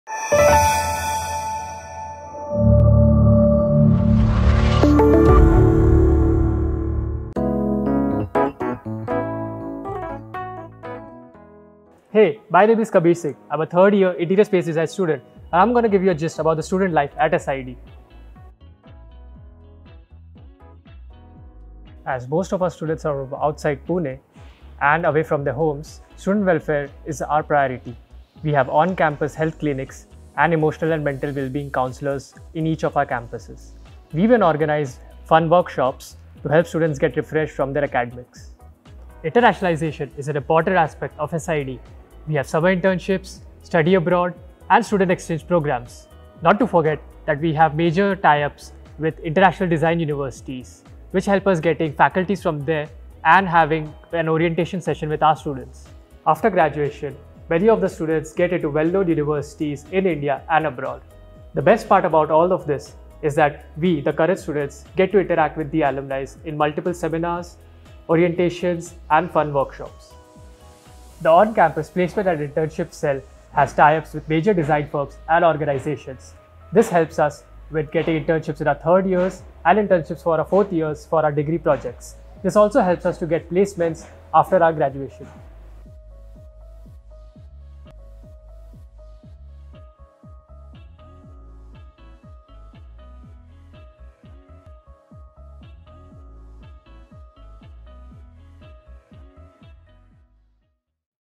Student Testimonial - Kabir Singh
When you are a student designer @ SID, life becomes so exciting! From expert faculty, design counselors to guide you, exhibitions, cultural festivals, and more, there are so many ways to DesignYourDestiny! Hear it from a 3rd Year Interior Space Design student @ SID.